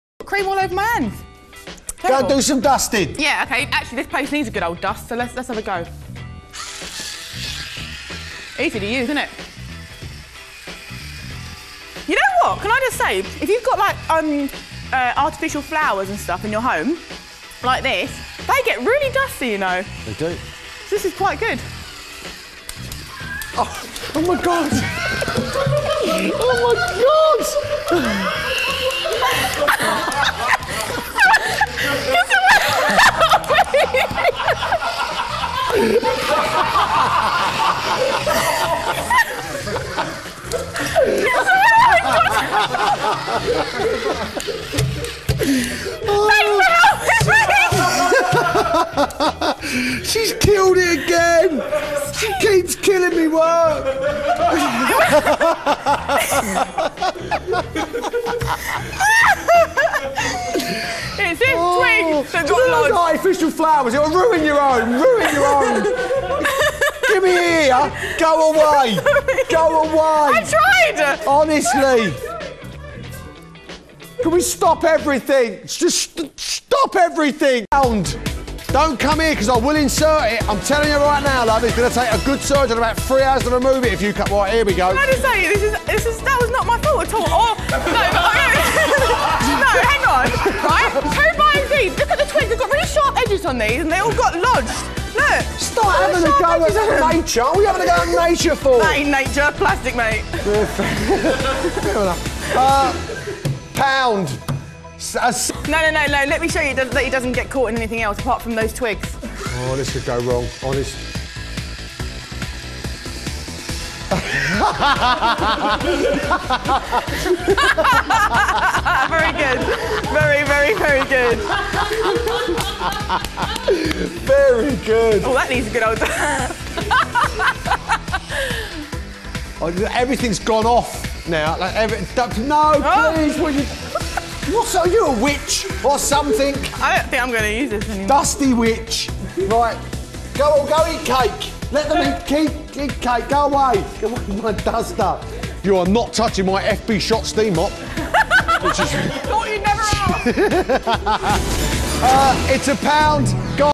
Marina Berry attacks a twig! (TV Blooper) original!
REC on 17/1/2013
A battery operated feather duster and Marina the co-host meets a plastic twig! And the rest is history.
That is an offensive weapon marina By Tel